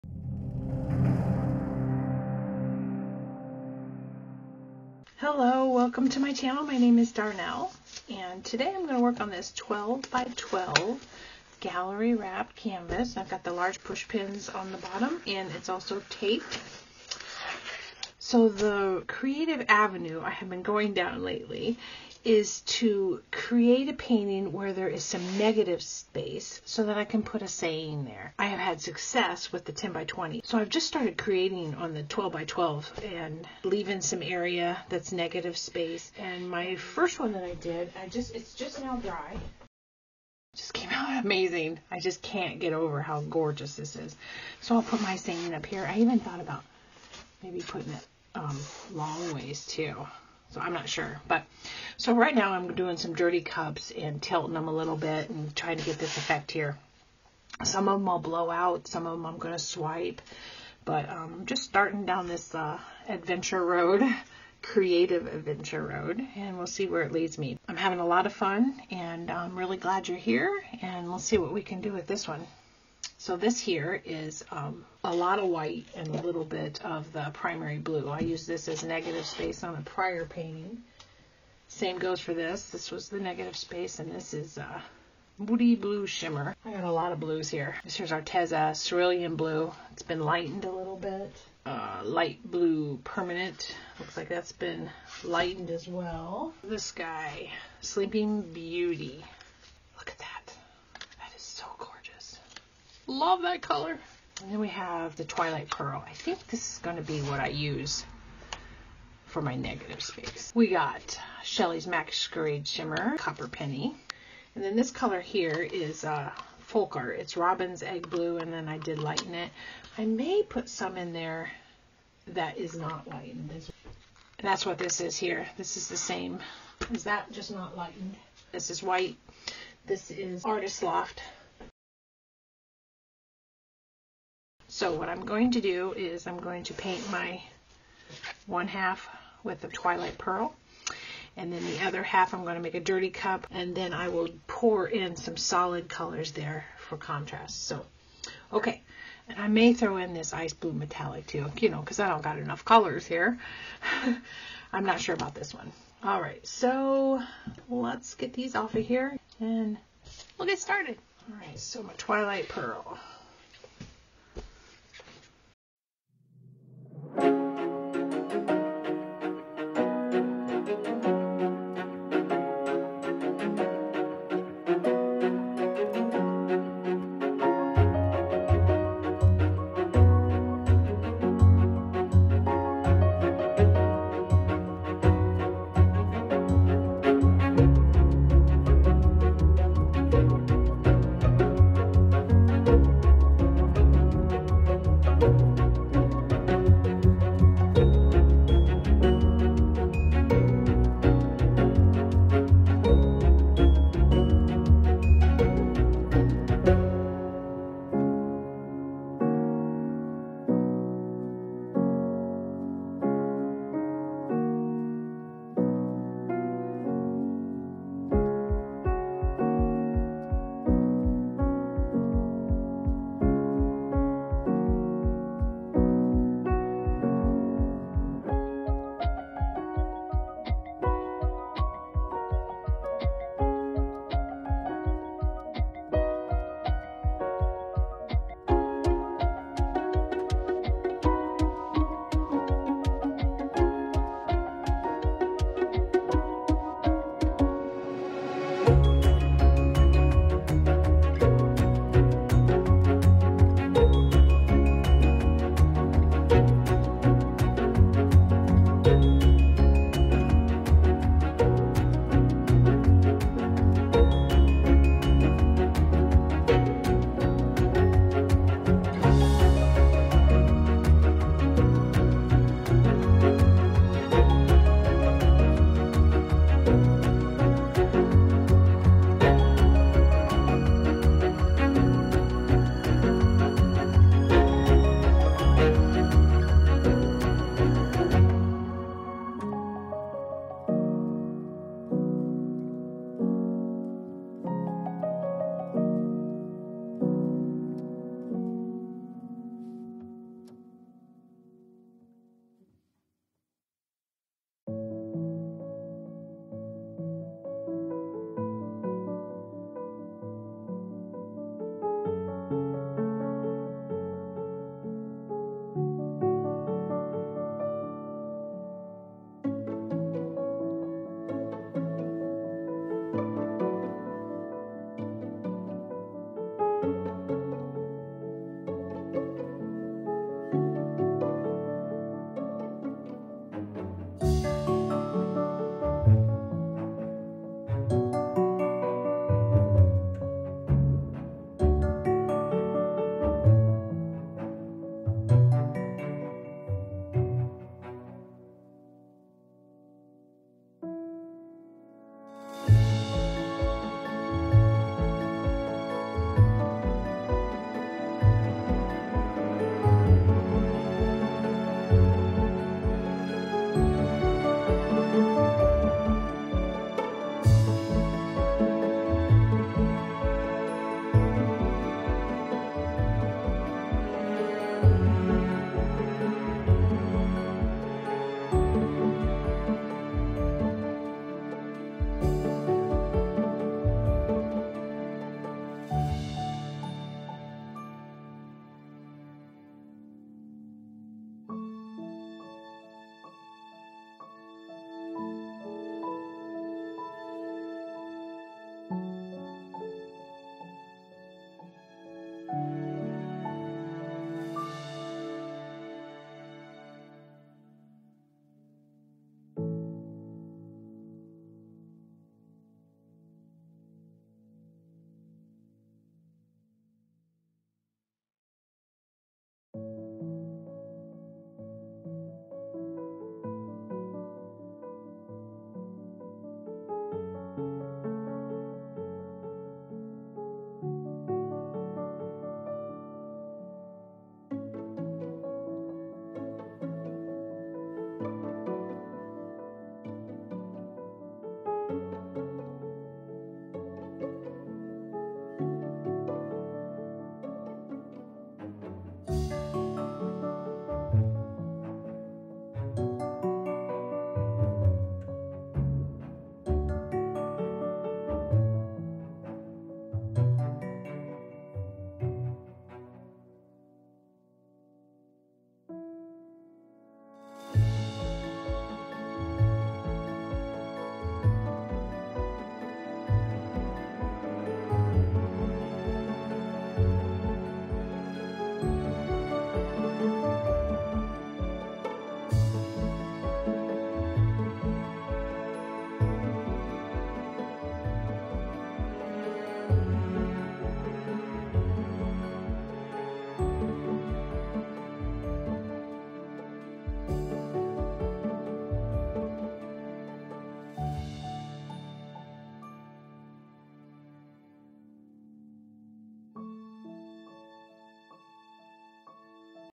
Blue 12x12 Dirty Pour with inspirational embellishment/ Fluid Art/ Acrylic Pouring (155)
Blue 12x12 Dirty Pour with inspirational embellishment/ Fluid Art/ Acrylic Pouring

12x12 Gallery Wrapped Canvas

Today I painted half of my canvas with a variety' of blues along with a dirty cup and the other half I painted with only one color. Once dry, I then added a vinyl saying then varnished over it all. I love how it came out~

Paint mixture: 1 part paint, 1 part Glue all / 1 part Pouring medium, 2-3 parts Floetrol

Please take precautions and paint in a well-ventilated area.

God Bless

Darnell